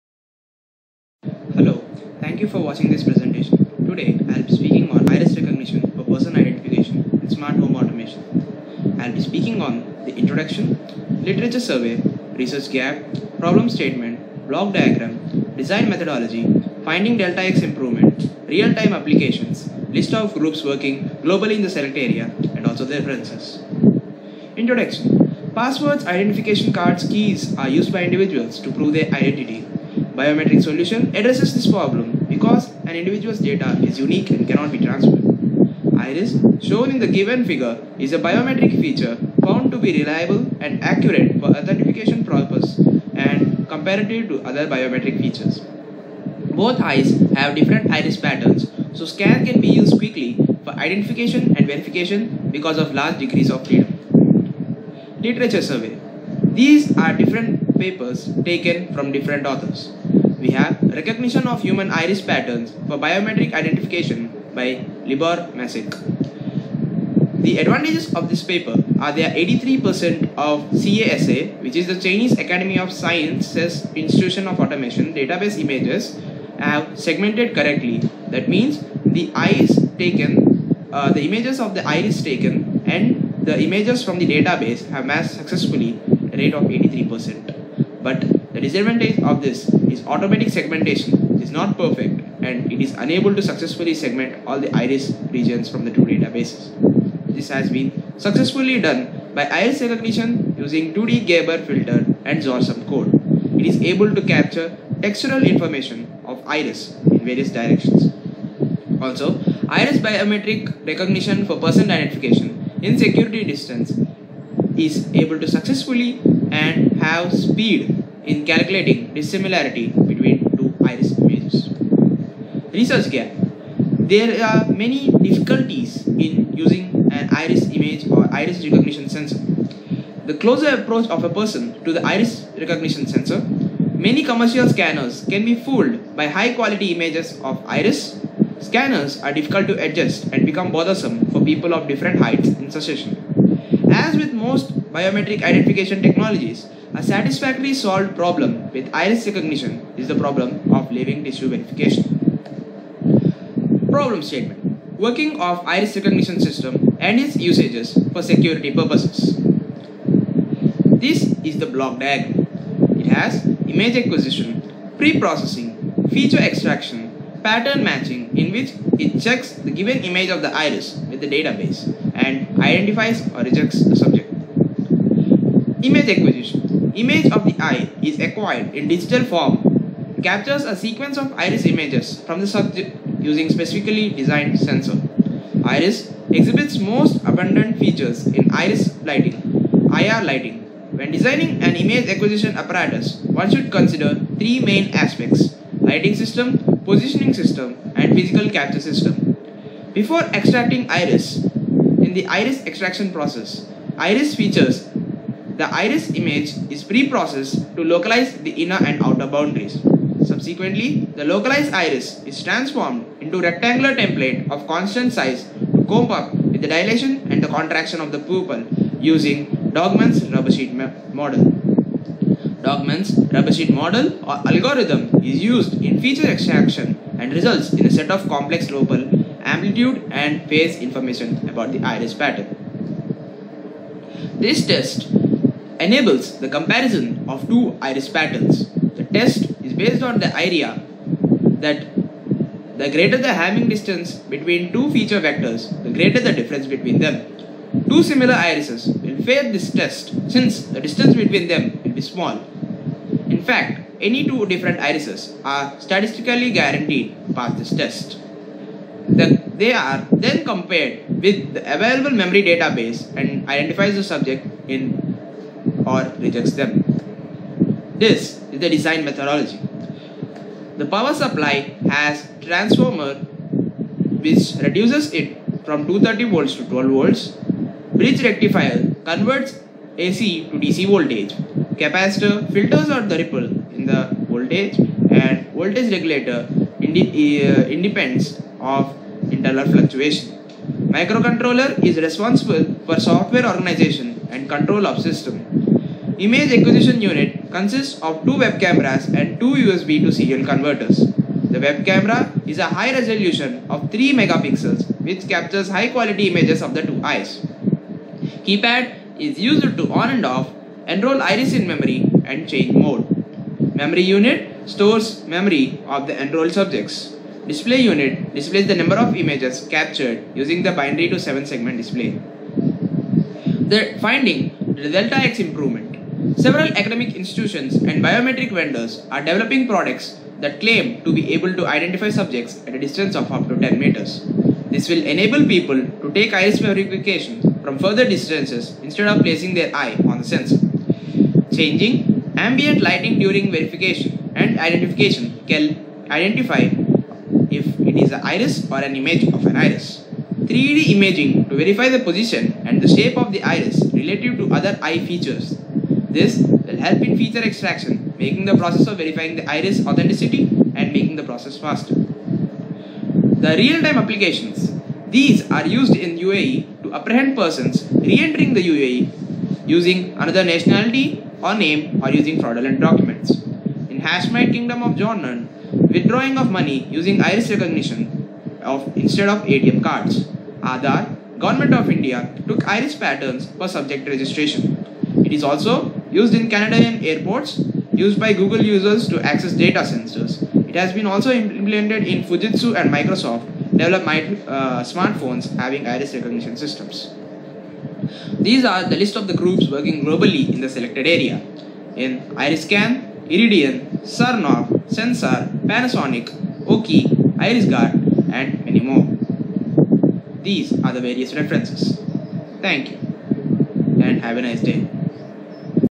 Iris recognition in Smart Home Automation
This video describes the methodology involved, working principle, implementation of iris recognition system.